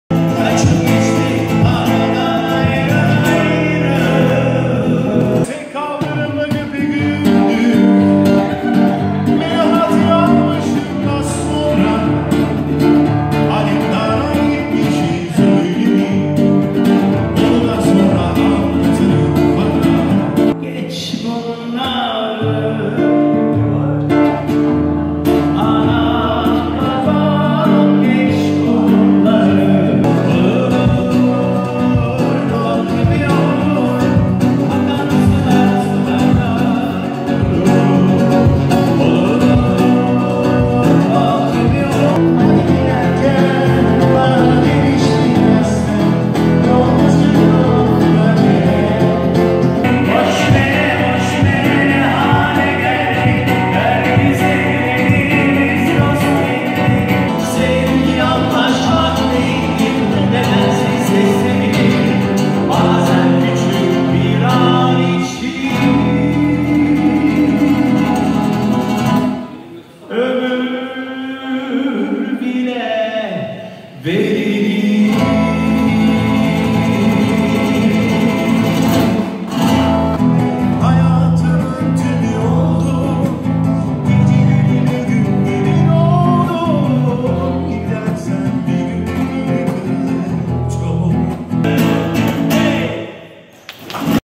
Canlı Müzik Nostaljik Şarkılar Ahmet Cemal & Asil Arman
Joya Del Mar Boutique Hotel de Nostalji şarkılar ile müzik keyfi..